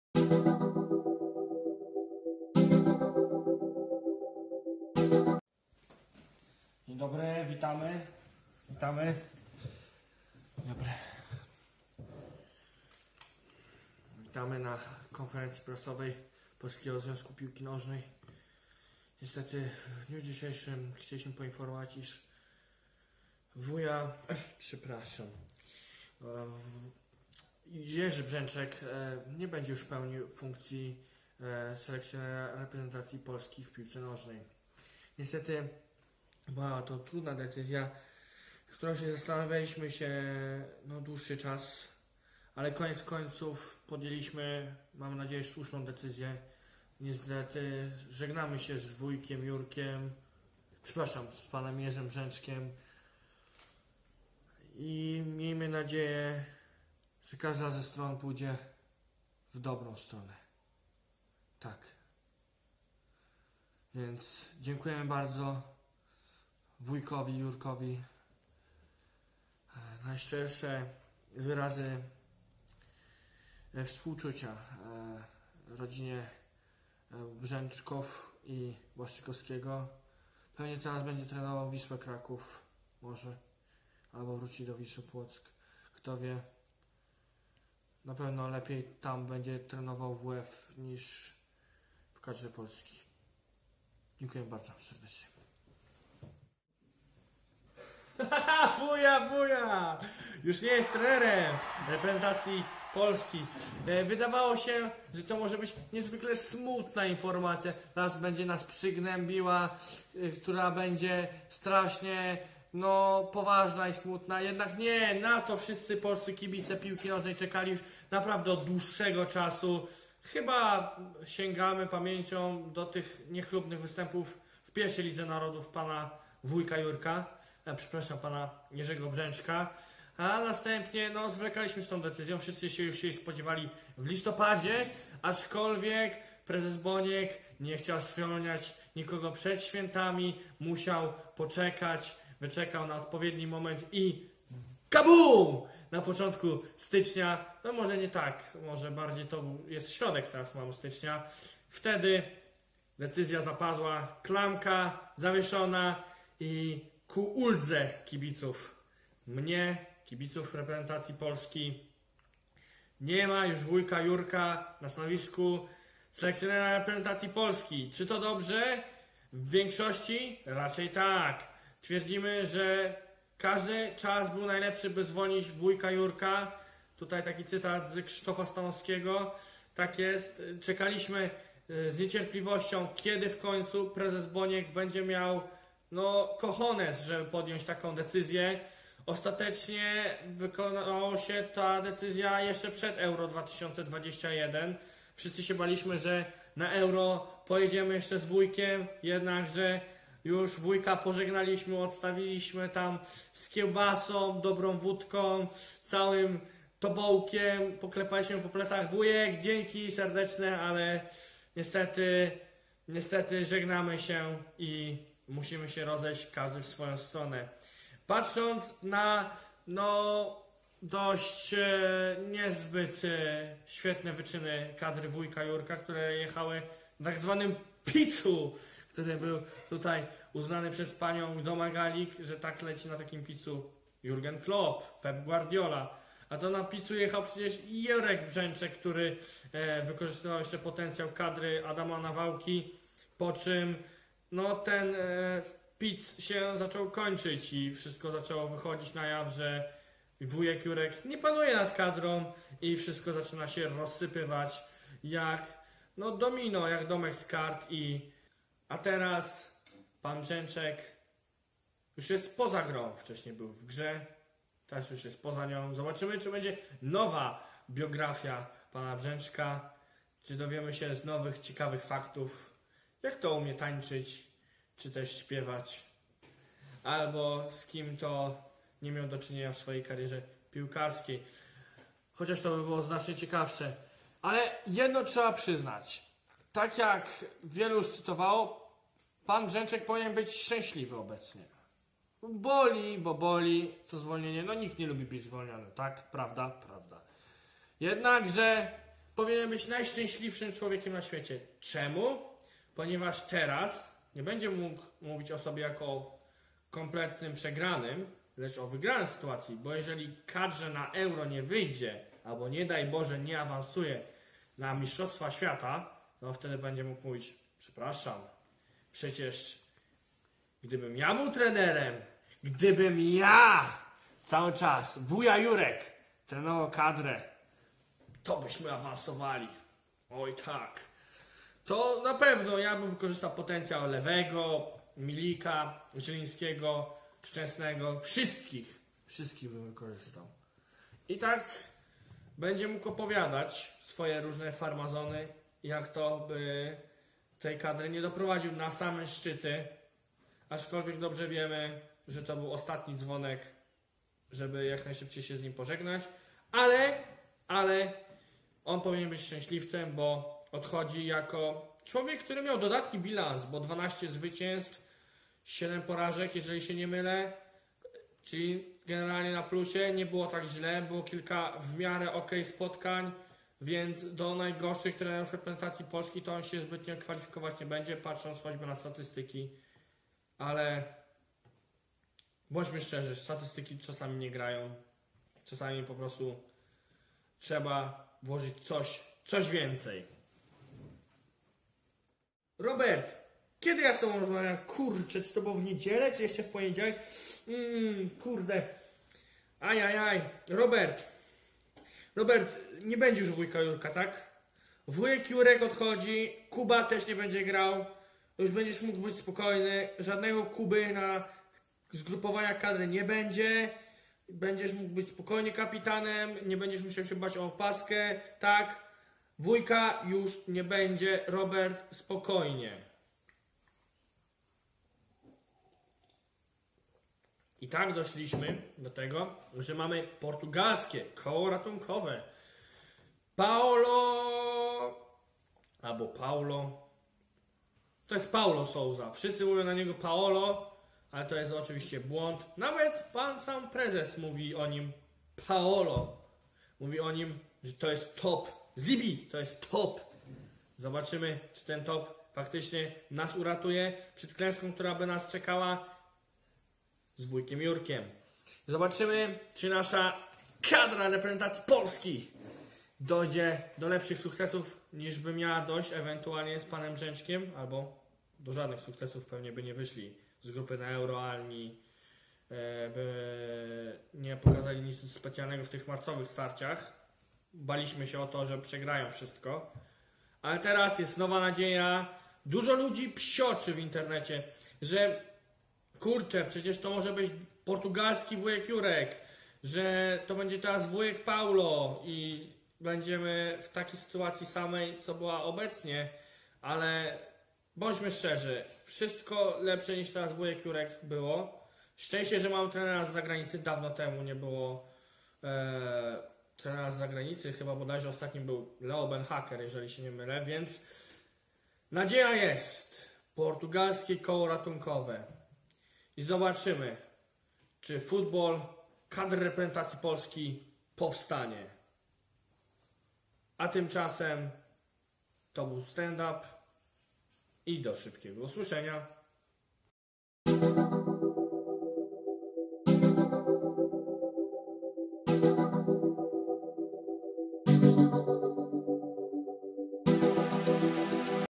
Football Stand Up: Poza Grą
Zapraszamy na Football Stand Up, gdzie brany jest pod lupę kontrowersyjny, ciekawy temat piłkarski i najczęściej będzie obracany w luźny żart.
Tematem pierwszego wydania Stand Upu jest zwolnienie Jerzego Brzęczka oraz ogłoszenie nowego selekcjonera reprezentacji Polski.
Piłko Nożno Powstań!

W Stand Upie wystąpił Kacper Wadych.